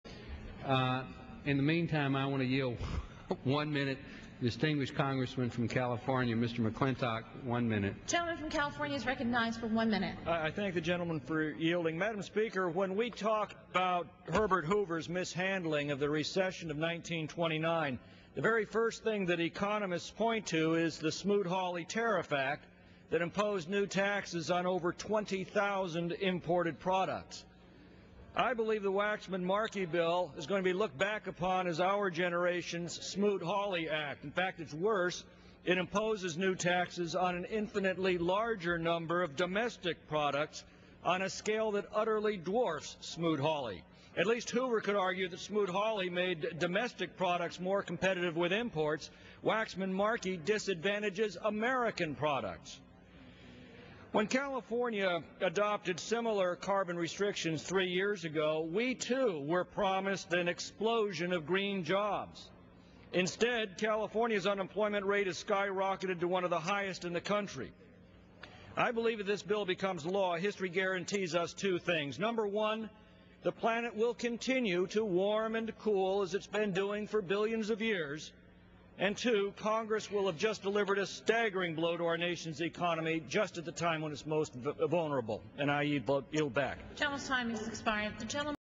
Rep. Tom McClintock - "No" on Cap and Trade Legislation
Congressional Debate on H.R. 2454 the "American Clean Energy and Security Act"